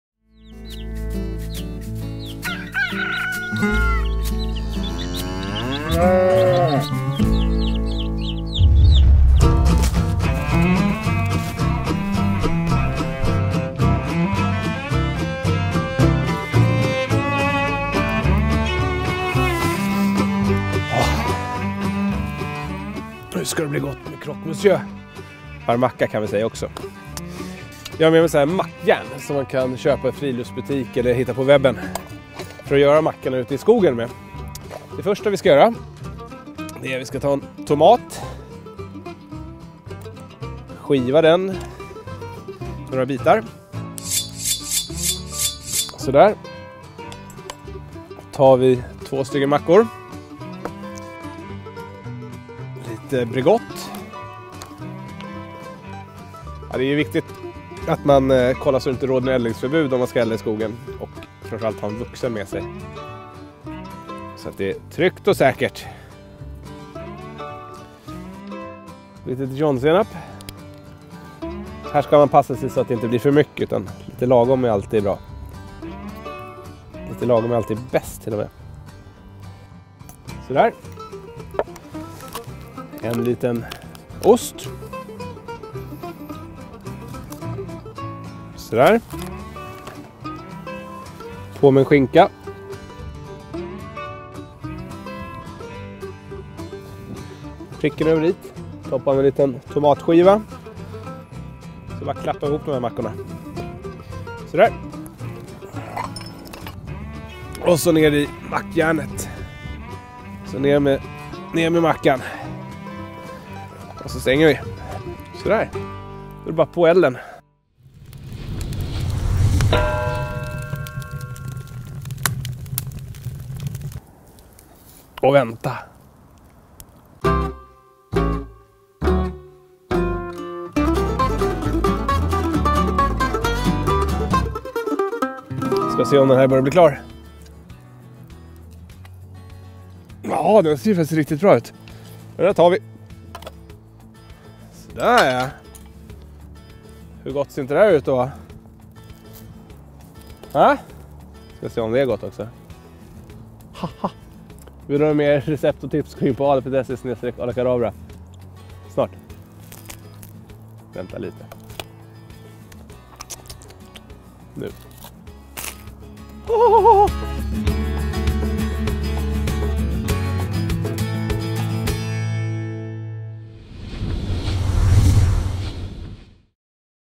Gör varma mackor till picknicken | Arlakadabra
Grilla jättegoda varma mackor - både hemma och ute i naturen! Arlakadabra visar hur du gör. Perfekt till picknicken och som mellanmål!

Kolla in våra andra spännande videor i vår Baka och laga mat med Arlakadabra-spellista: